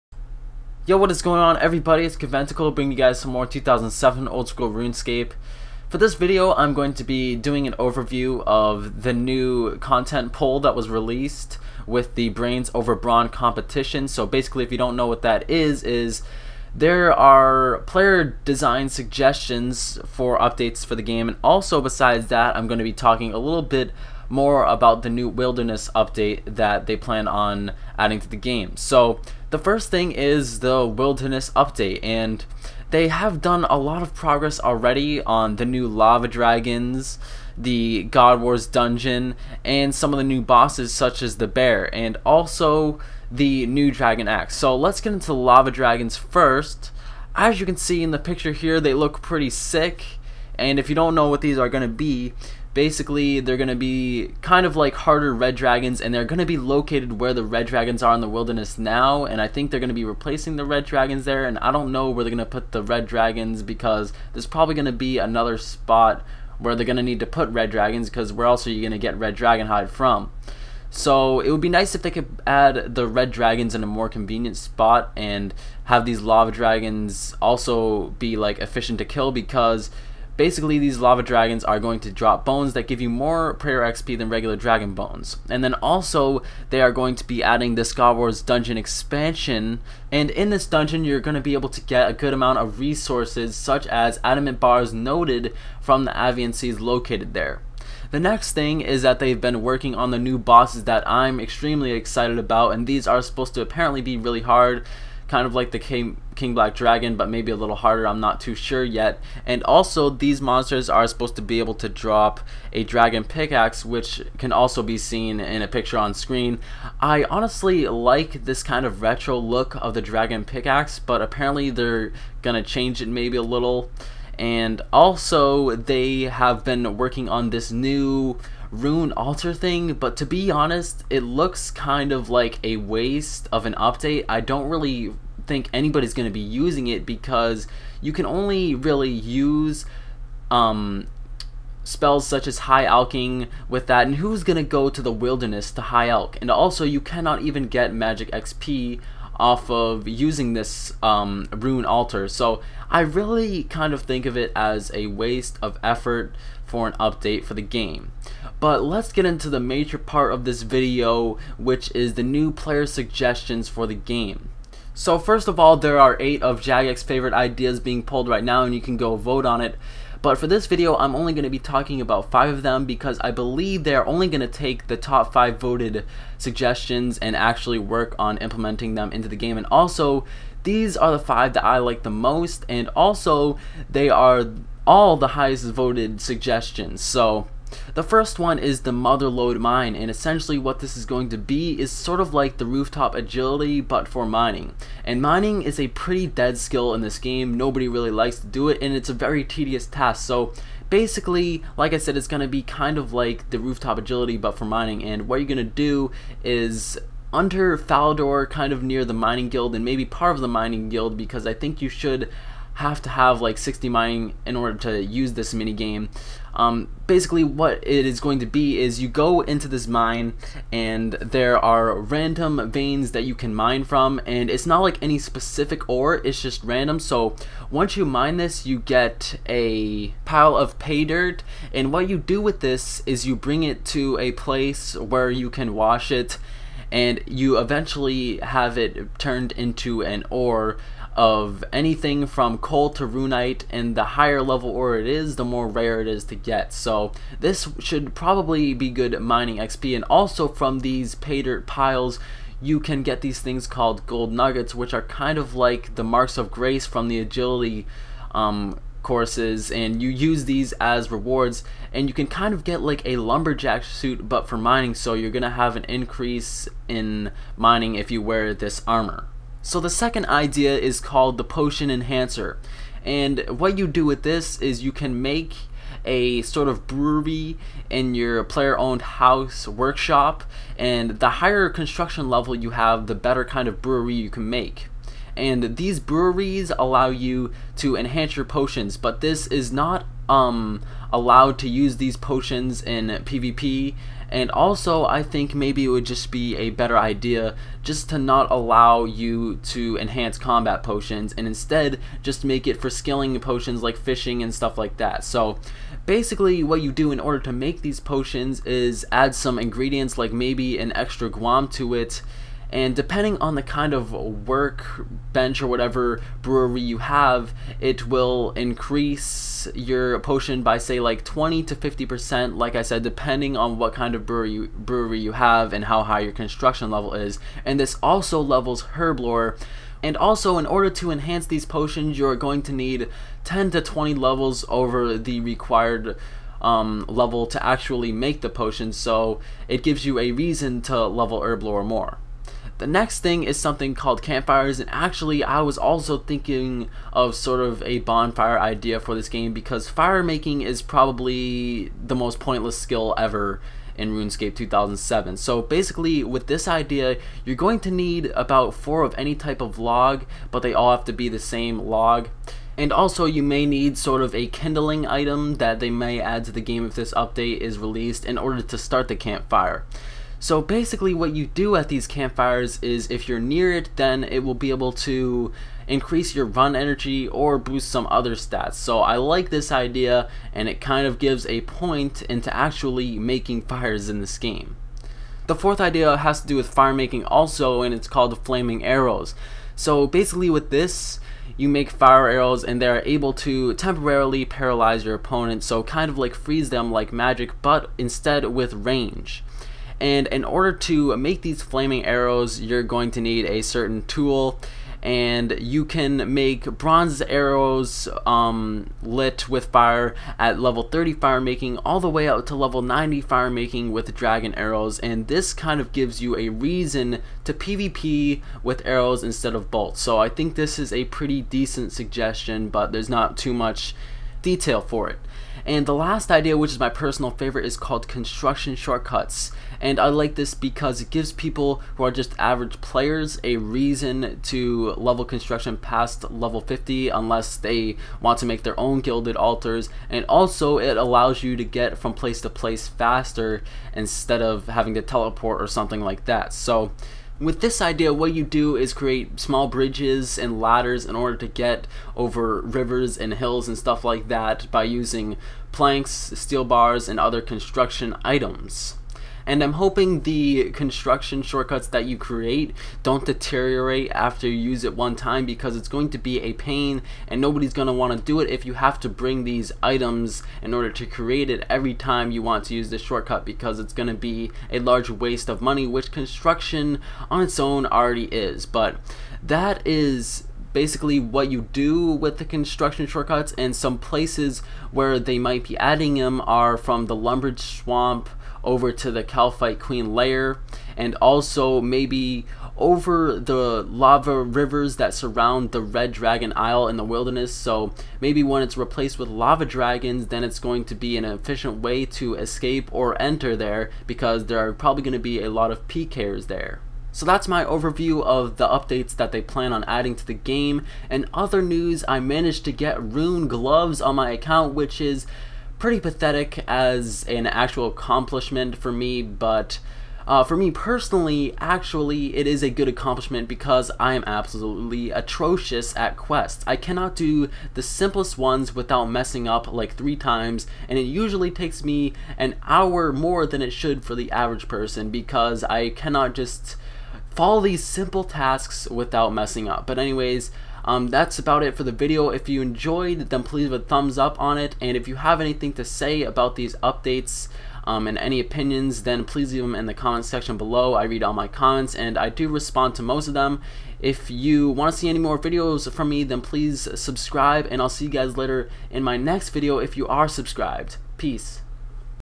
2007 Oldschool Runescape: Overview of New Player Designed Content and Other Updates!
Most of these updates are coming in Mid-March, I cannot wait!